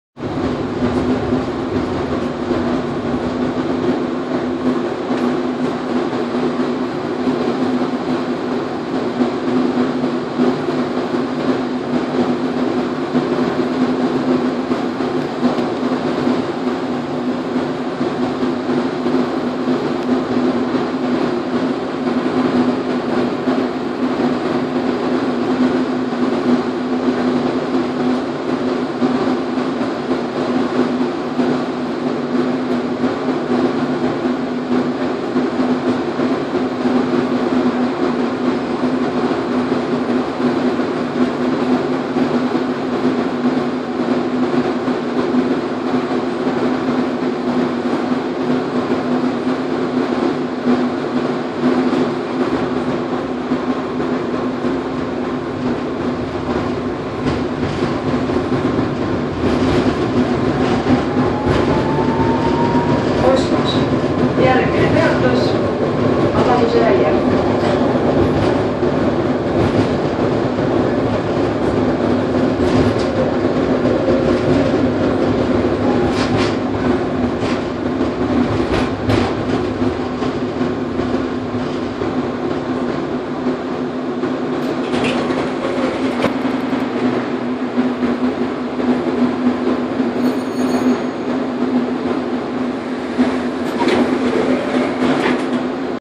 Tallinn tram ride in 2015
Tram ride in Tallinn January 2015 with the old tram.